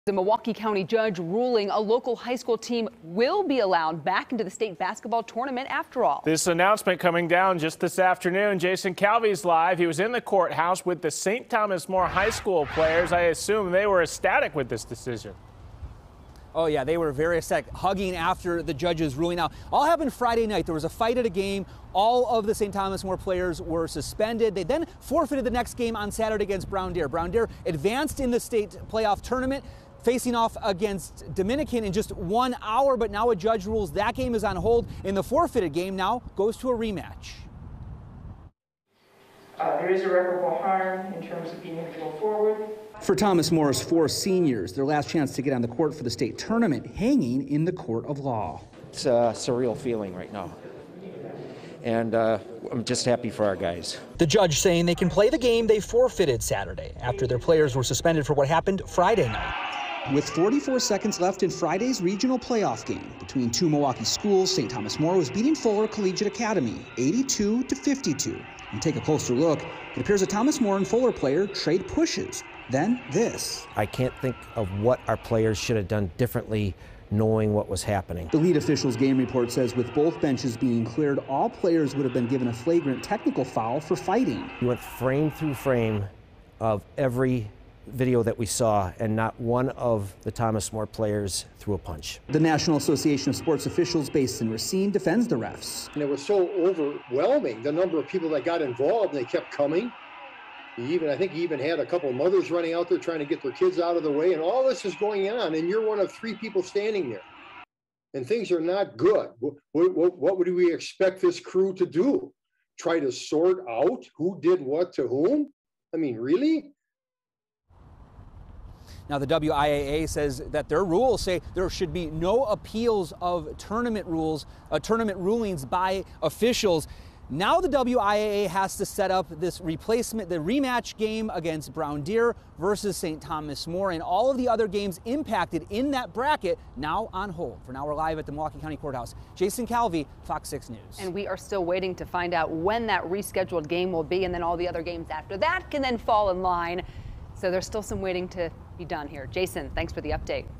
Thomas More High school appeals, team back on basketball court | FOX6 News Milwaukee
St. Thomas More High School in Milwaukee went to court on Wednesday, March 9 – fighting to get its team back on the basketball court – and the school is getting that chance.